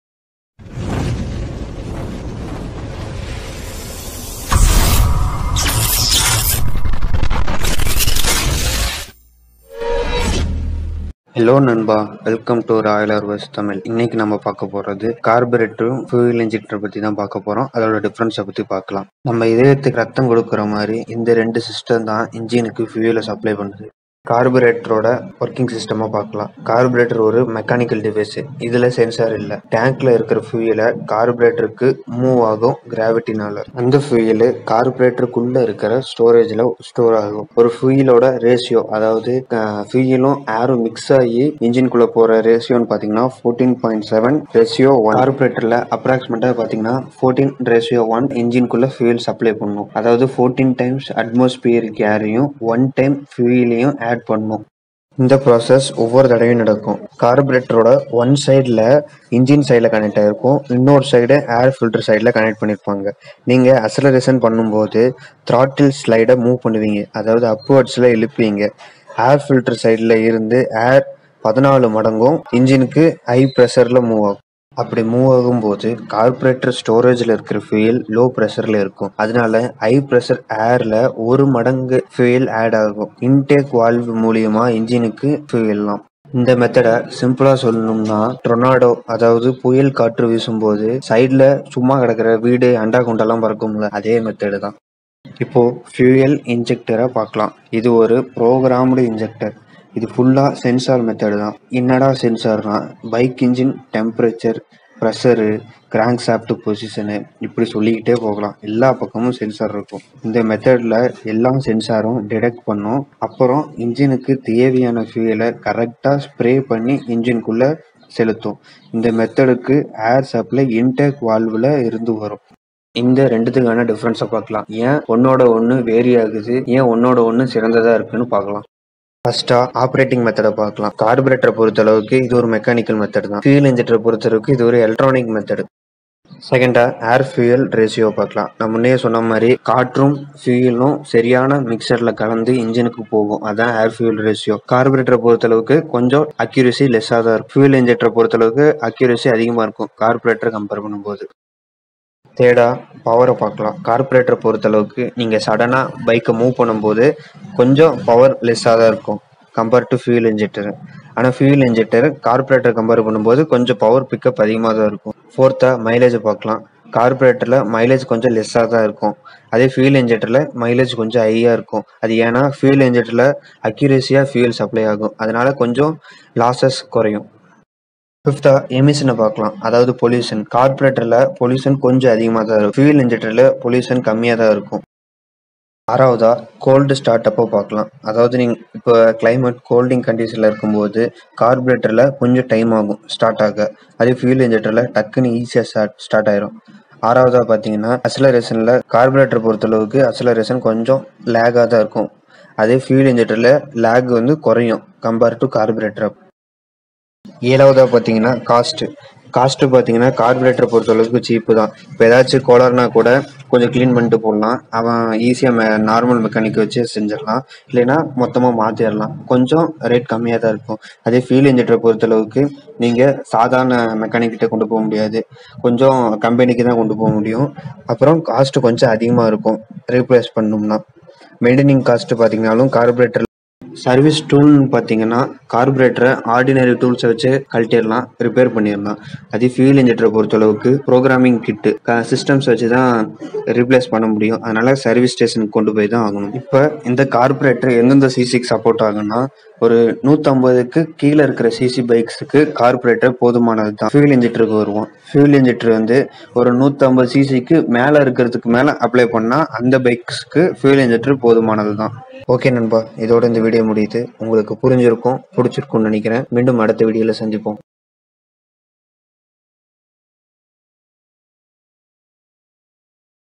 How CARBURETOR and FUEL INJECTOR works? || difference explained in tamil
carburetor VS fuel injector
difference and working condition explained in tamil